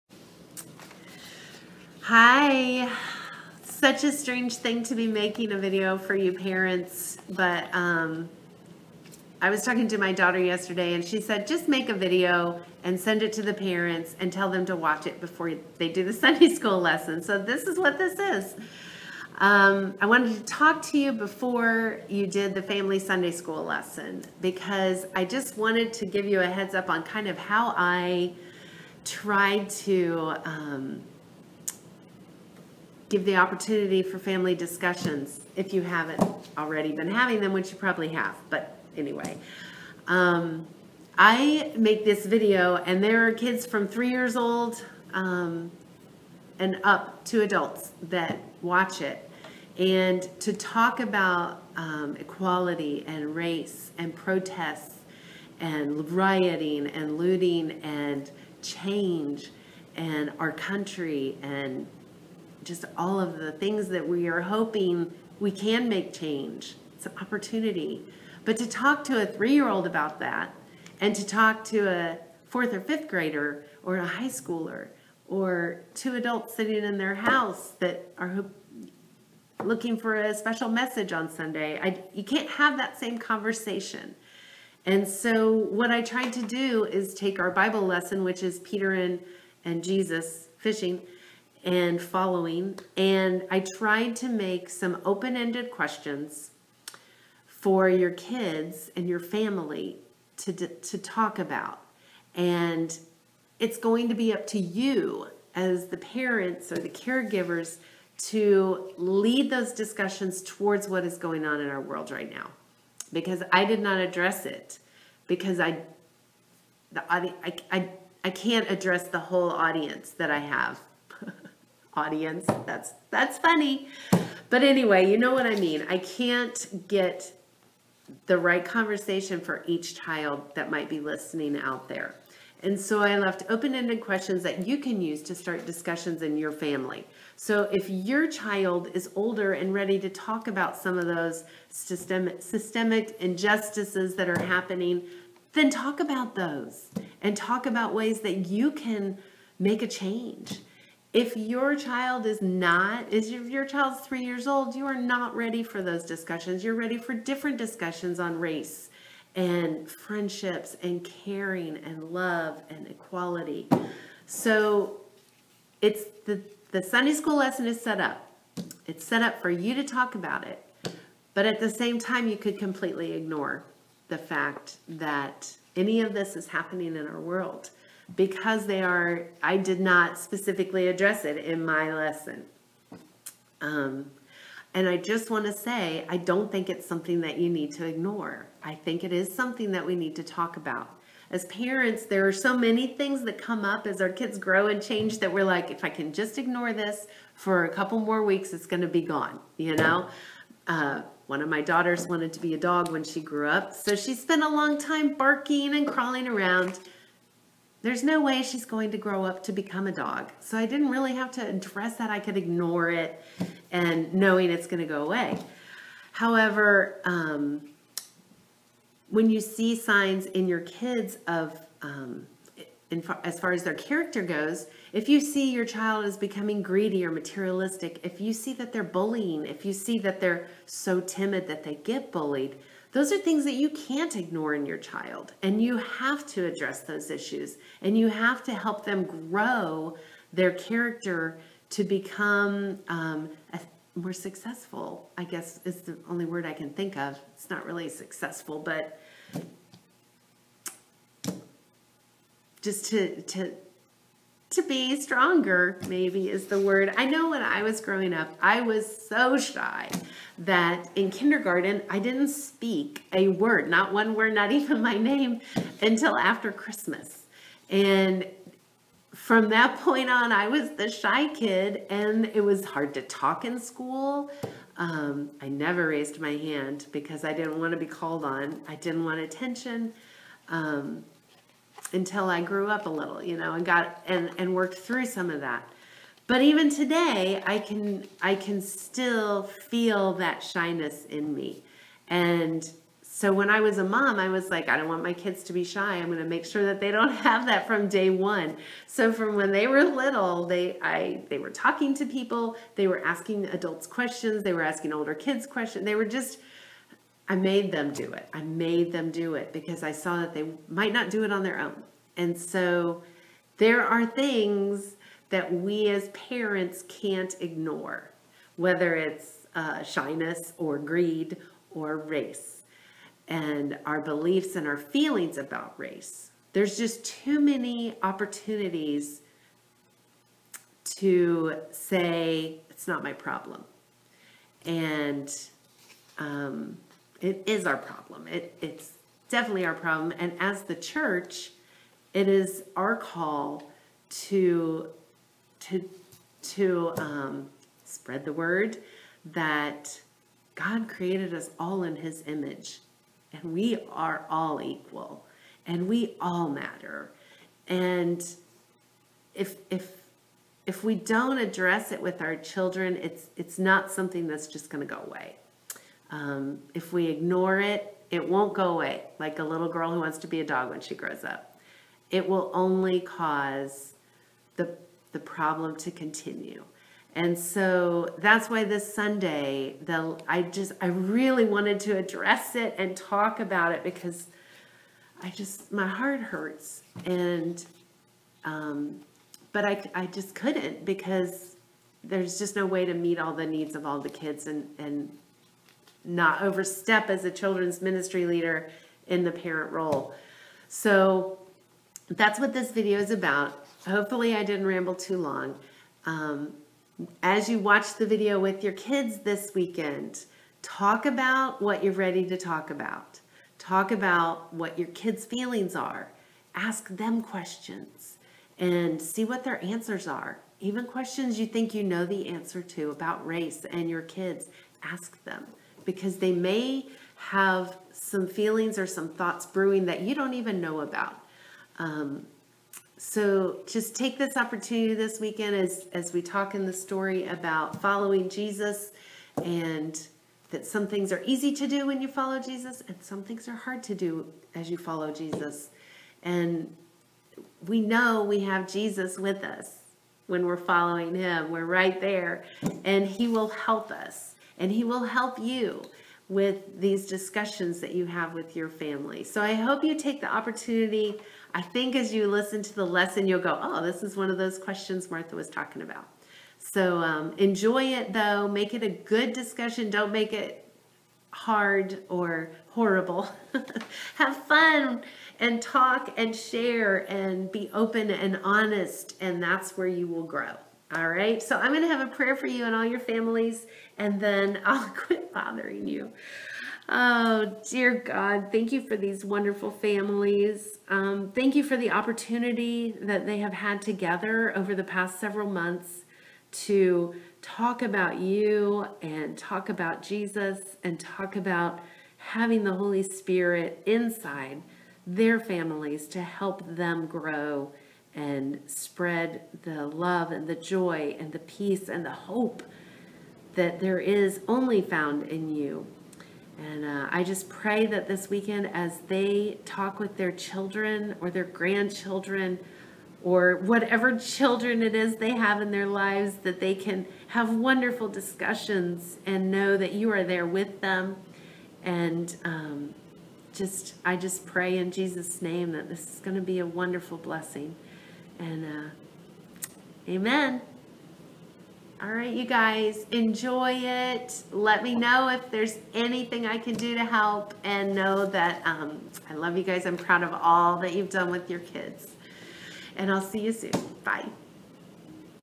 Parents & Caregivers
A message to Parents and Caregivers to help with your discussion time during the family Sunday lesson on Peter Fishing and Following Jesus.  It is recommended that you watch this video BEFORE you view the lesson with your family.